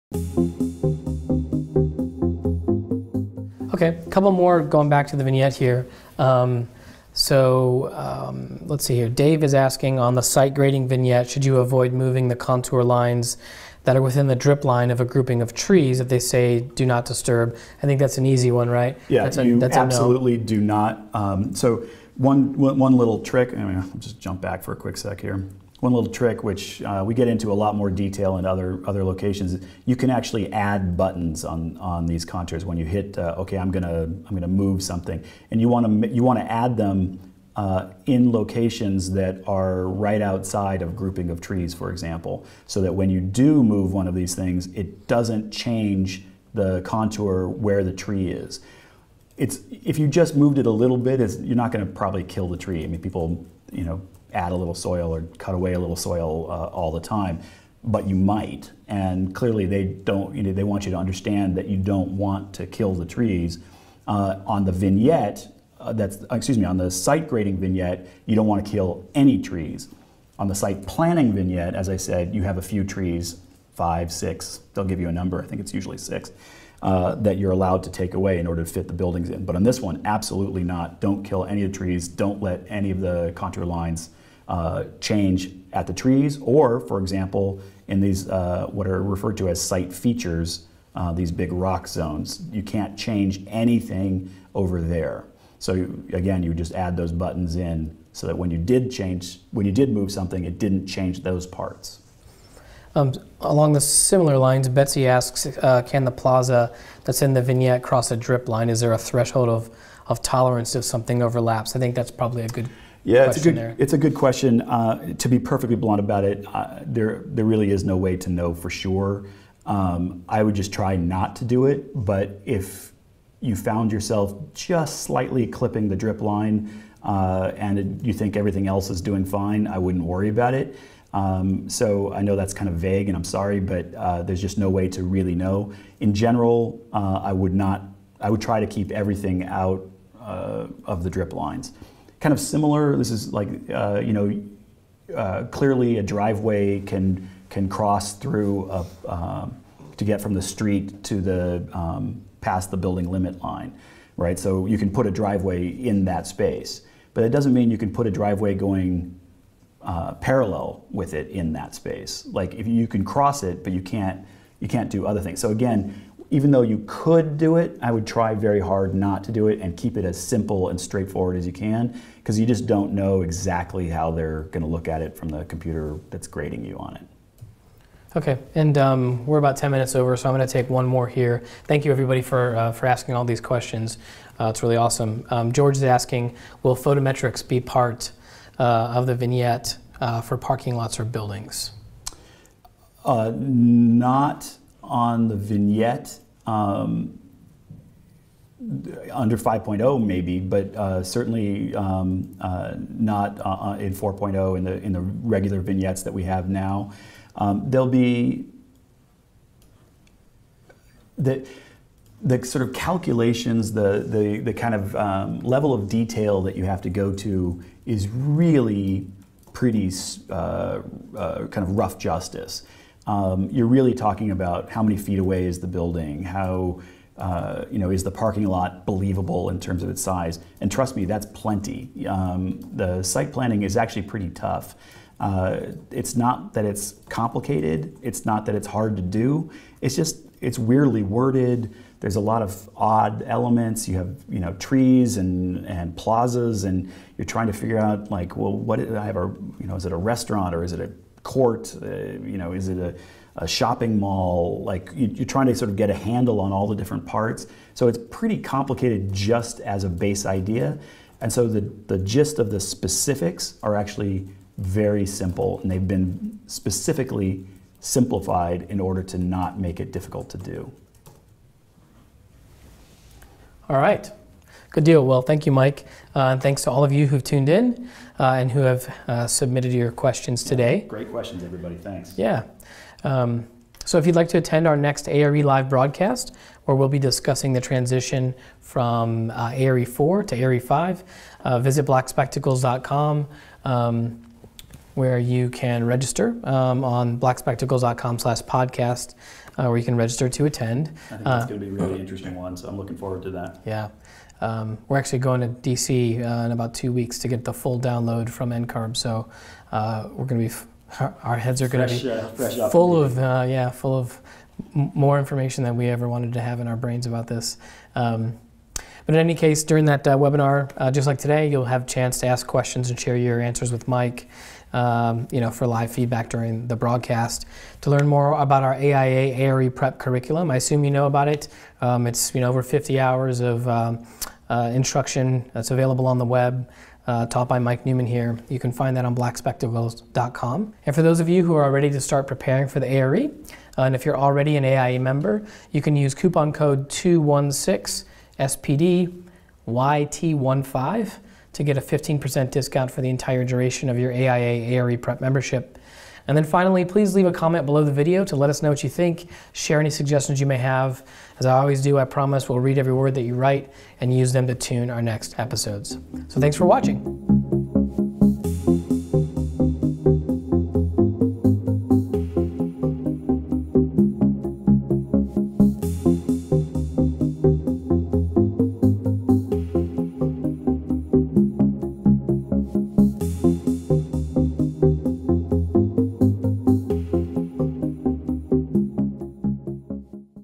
Question and Answer: Part 2 - Site Planning Review - Architect Registration Exam A.R.E. Webinar
ARE Prep powered by Black Spectacles-

Marc: Okay. A couple of more, going back to the vignette here. So let's see here, Dave is asking to avoid in the contour lines that are within the drip line of the grouping of trees. You can say do not disturb.

Mike: Yeah, you absolutely do not…so one little trick…let me just jump back for a quick sec here. One little trick which we get into a lot more detail in other locations is you can actually add buttons on these contours when you hit, okay, I'm going to move something. And you want to add them in locations that are right outside of grouping the trees for example. So that when you do move one of these things, it doesn't change the contour where the tree is. If you just moved it a little bit, you're not going to probably kill the tree, and people add a little soil and cut away a little soil all the time.

But you might, and clearly they want you to understand that you don't want to kill the trees. On the site grading vignette, we don't want to kill any trees. On the site planning vignette, as I said, you have a few trees, five, six, they'll give you a number. It's usually six that will allow [inaudible 01:21:29] in order to build the buildings. On this one, absolutely not. Don't kill any trees.

Don't let any of the contour lines change at the trees, or for example, in these, what are referred to as site features, these big rock stones. You can't change anything over there. So again, you just add those buttons in so when you did move something, it didn't change those parts.

Marc: Along the similar lines, Betsy asks, "Can the plaza that's in the vignette cross a drip line? Is there a threshold of tolerance in overlaps?" I think that's probably a good question.

Mike: Yeah, that a good question. And to be perfectly blunt about it, there really is no way to know for sure. I would just try not to do it. But if you found yourself just slightly clipping the drip line, and you think everything else is doing fine, I wouldn't worry about it. So I know that's kind of vague, and I'm sorry, but there's just no way to really know. In general, I would try to keep everything out of the drip lines. It's kind of similar, this is like, you know, clearly a driveway can cross through to get from the street to past the building limit line. Right?

So you can put a driveway in that space. But it doesn't mean you can put a driveway going parallel with it in that space. Like if you can cross it but you can't do other things. So again, even though, you could do it, I would try very hard not to do it and keep it as simple and straight forward as you can. Because you just don't know exactly how they're going to look at it from the computer that's grading you on it.

Marc: Okay. And we are about 10 minutes over. So I'm going to take one more here. Thank you, everybody for asking all these questions. It's really awesome. George is asking, "Will photo metrics be part of the vignette for parking lots or buildings?

Mike: Not on the vignette. Under 5.0 maybe, but certainly not in 4.0 in the regular vignettes that we have now. The calculations, the level of detail that you have to go to is really pretty kind of rough justice. You're really talking about how many feet away is the building, and is the parking lot believable in terms of its size. And trust me, that's plenty. The site planning is actually pretty tough. It's not that it's complicated. It's not that it's hard to do. It's just, it's weirdly worded. There's a lot of odd elements, you have trees, and plazas, and you're trying to figure out is it a restaurant or is it a court, you know, is it a shopping mall? You're trying to sort of get a handle on all the different parts. So it's pretty complicated just as a base idea. And so the gist of the specifics are actually very simple, and they've been specifically simplified in order to not make it difficult to do.

Marc: All right. Good deal. Well, thank you, Mike. Thanks to all of you who tuned in and submitting your questions today.

Mike: Great questions, everybody. Thanks.

Marc: Yeah. So if you'd like to attend our next ARE Live podcast where we'll be discussing the transition from ARE 4 to ARE 5, visit blackspectacles.com where you can register where you can register to attend.

Mike: It's going to be a really interesting one. So I'm looking forward to that.

Marc: We're actually going to DC in about two weeks to get the full download from [inaudible 1:25:20]. So our heads are going to be full…

Mike: Fresh out of the meeting.